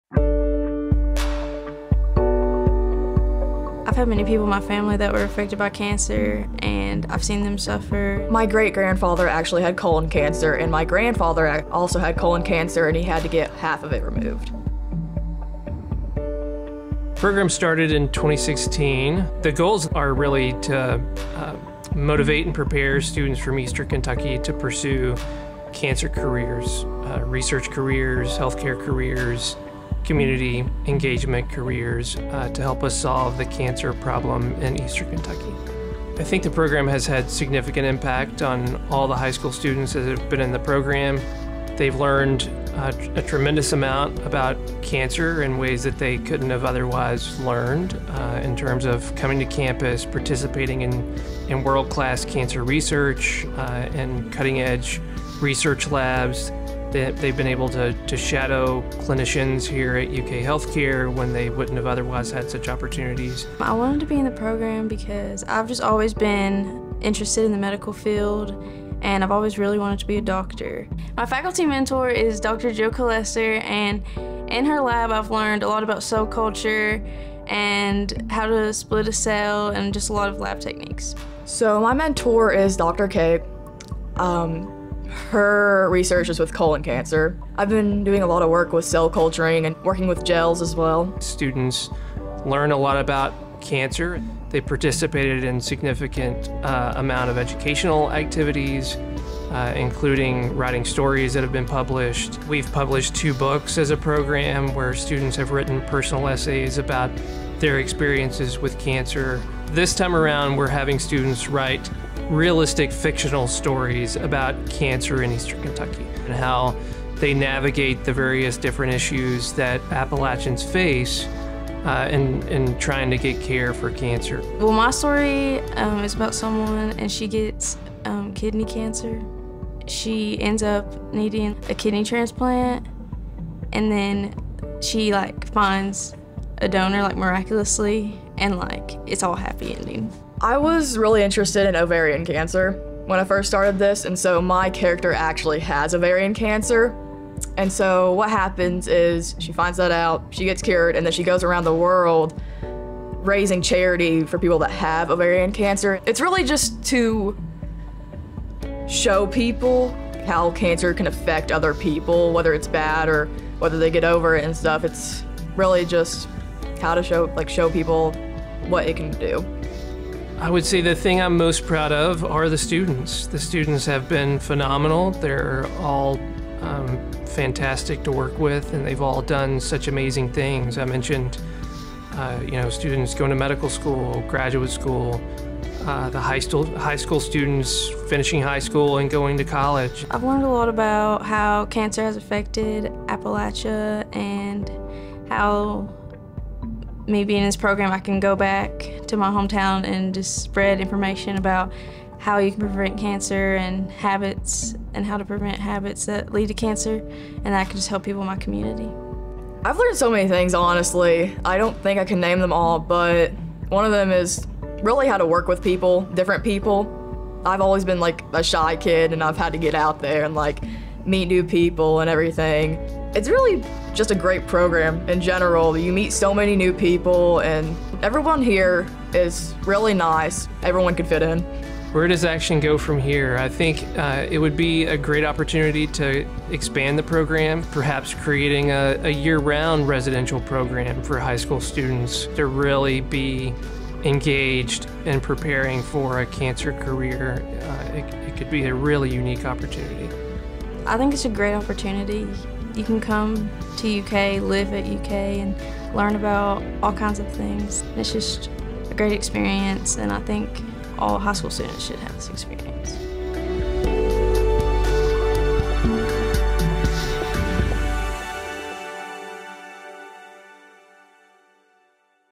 Markey’s ACTION program gets $2 million grant renewal from NCI
The University of Kentucky Markey Cancer Center’s Appalachian Career Training in Oncology (ACTION) program has received $2 million in renewed funding from the National Cancer Institute (NCI) that will support the program over the next five years.

Launched in 2016, ACTION is a cancer education and training program aimed at high school and undergraduate students from Appalachian Kentucky. This region grapples with the highest cancer incidence and mortality rates in the nation, primarily due to health, socioeconomic and educational disparities.